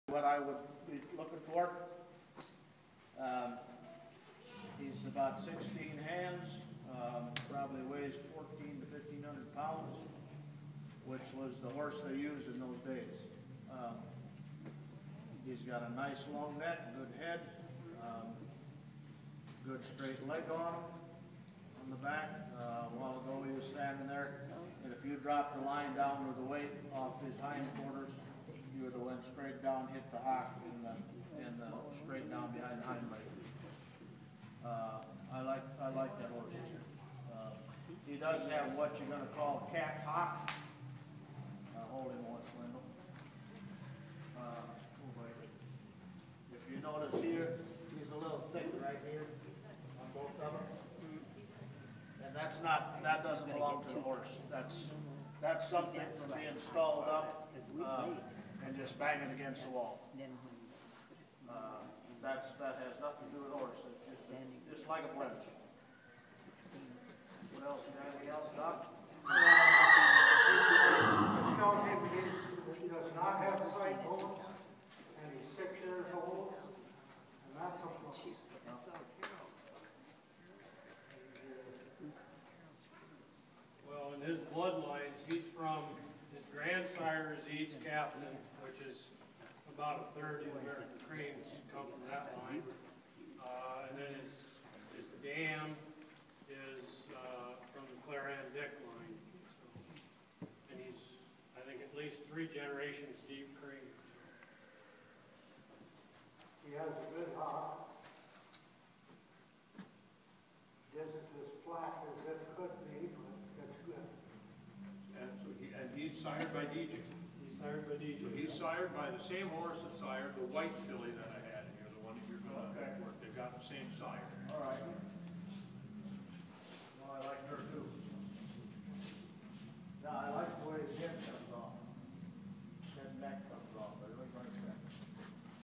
American Cream Draft Horse Show
This national horse show came to Spencer, IA for the first time. Learn what they're looking for in the cream Draft Breeds.